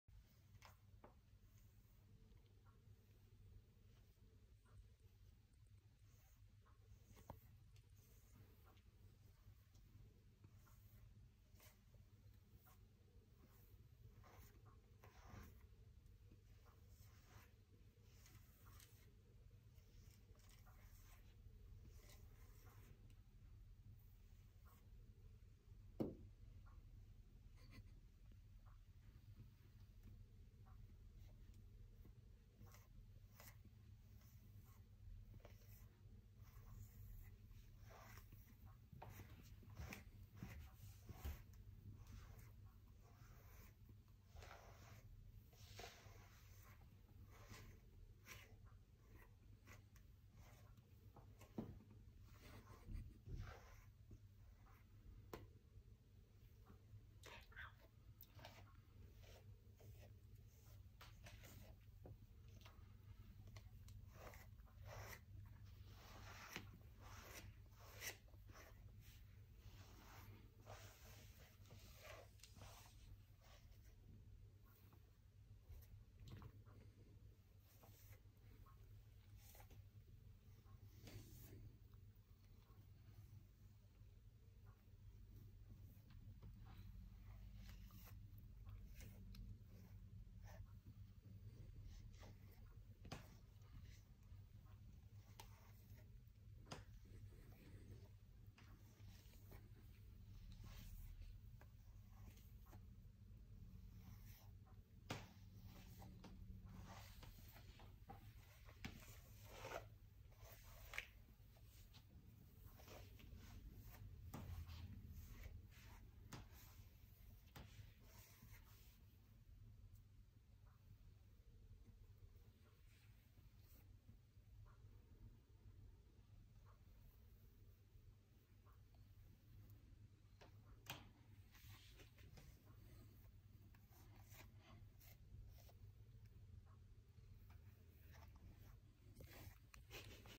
The Daily Grooming of Jinx HDR 4K 60 FPS
Here you see the daily grooming of our maine coon.